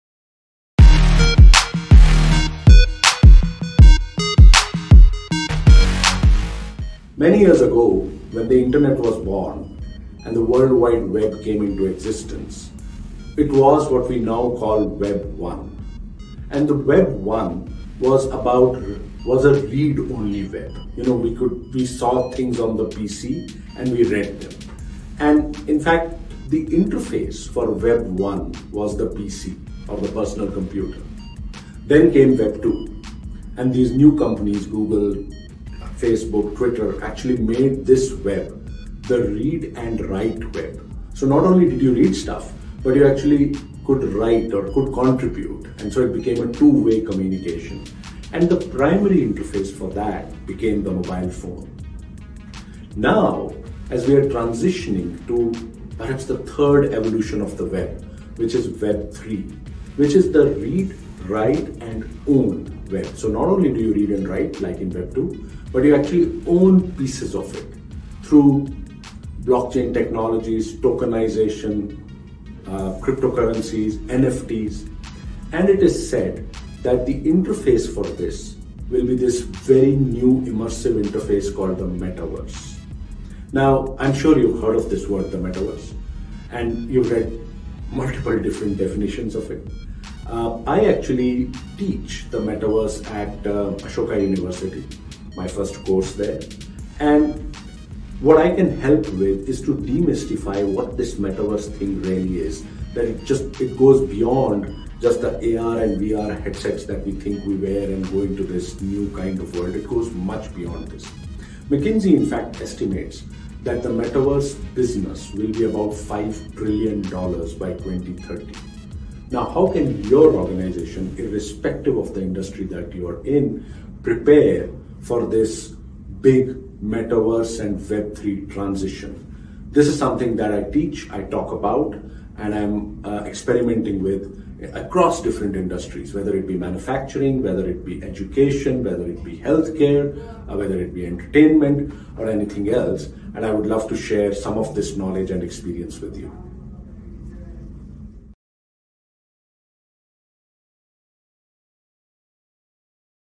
Jaspreet Bindra | For Better or for Verse: Understanding Metaverse & Web3
How can your organization prepare for the Metaverse and Web3 transition? In his session, aptly titled “For Better or for Verse”, DigitalTransformation expert Jaspreet Bindra talks about what these concepts mean for your business, how they can transform it, and how to prepare for this revolution.

A leading voice on the technologies that are changing our world, Jaspreet advises businesses on Digital Transformation and DigitalTechnologies like AI, Blockchain, Web3, and the Future of Work.

Know more. Listen to this conversation starter from the TOSB podcast where Jaspreet breaks down the basics of Metaverse. Talk to our team to engage with him for your organization.